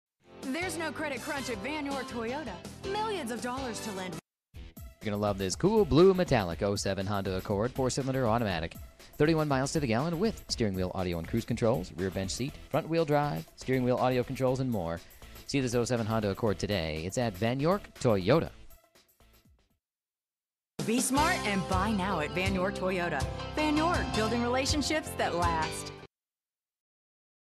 Used 2007 Honda Accord LX Greensboro, Winston Salem High Poi
Used 2007 Honda Accord LX Greensboro, Winston Salem High Point, Kernersville NC Vann York Toyota, imagine driving this Cool Blue Metallic 2007 Honda Accord, equipped with a 4 Cyl. engine and an automatic transmission with 97,993 miles. enjoy an exceptional 31 miles to the gallon on this great car with features like on steering wheel audio and cruise controls, rear bench seat, front wheel drive, steering wheel audio controls, and much more. Enjoy the drive and have peace of mind in this 2007 Honda Accord. see us at Vann York Toyota today!

miles- 97,993

vin- 1HGCM56377A094383

500 Eastchester Dr.